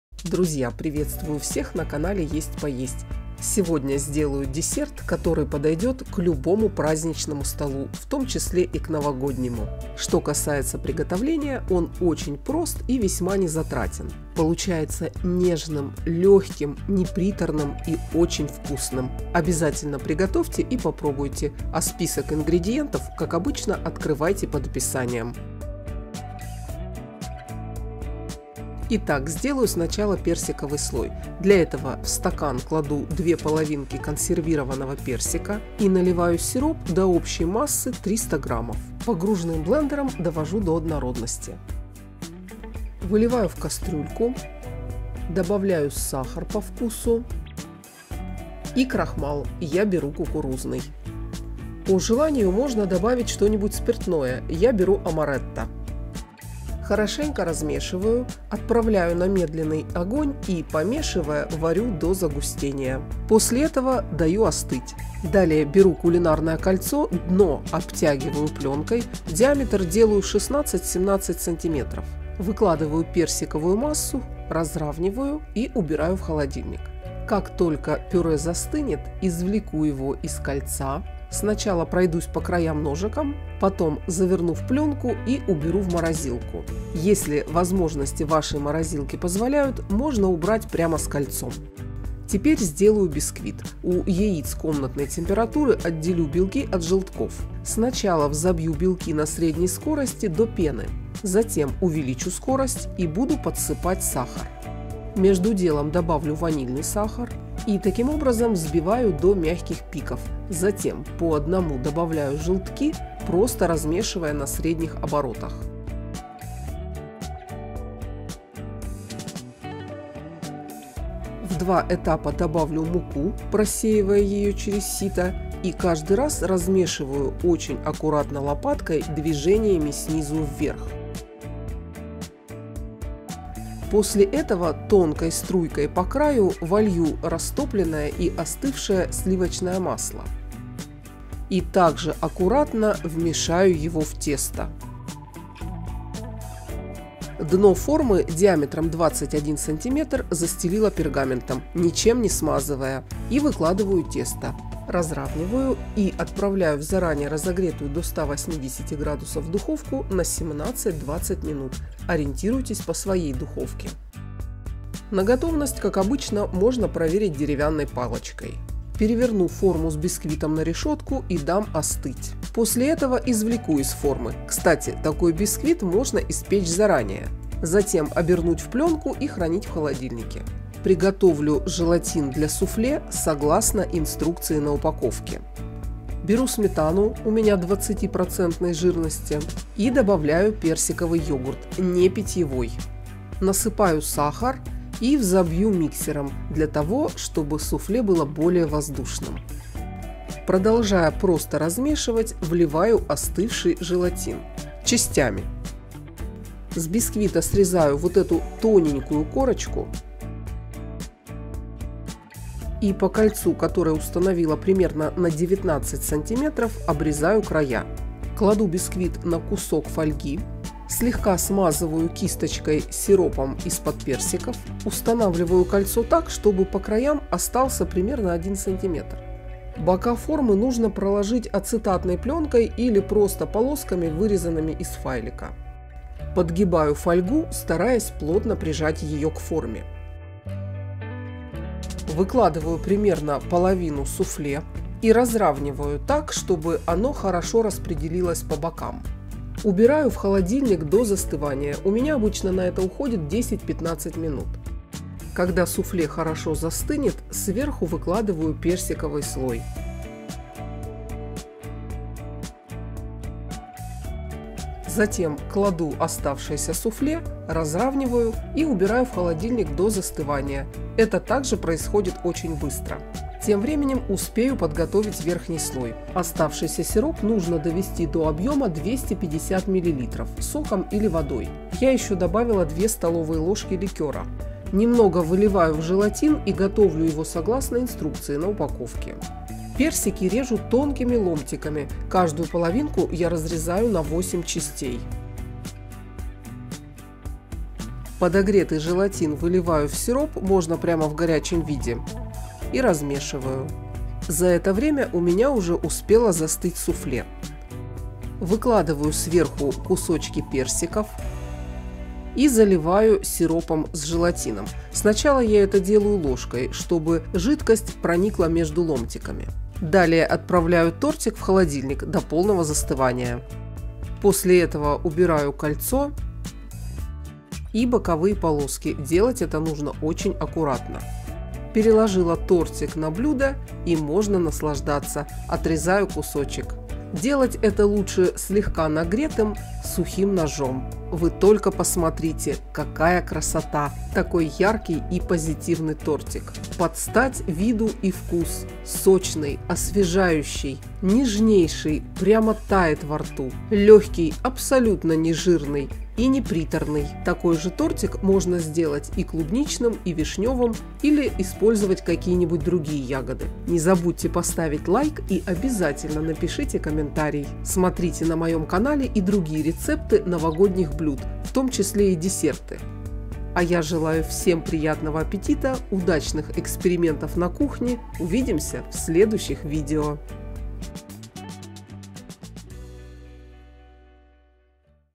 Простой в приготовлении, нежнейший торт с персиками.
Такой торт приготовить очень просто. Получается он нежнейшим, сочным и освежающим. Не жирный, не приторный - просто идеальный десерт. На основе этого рецепта можно сделать любой другой ягодный торт.
ИНГРЕДИЕНТЫ
Понадобится 1 банка консервированных персиков(половинками) -800г.
Тесто:
яйца 2 шт (С0 или С1),
ванильный сахар.
Крем-мусс:
сметана 20-26% - 400 г,
йогурт (не питьевой) 350 г,
сахар 80 г или по вкусу,
Персиковый слой:
персики 2 половинки,
сироп - до общей массы 300 г,
сахар 50 г,
крахмал кукурузный 35 г,
ликер(по желанию) 2 ст.л.
Верхний слой:
персики,
сироп+ вода - до общего объема 250 мл(при желании - ликер 2 ст.л.)  .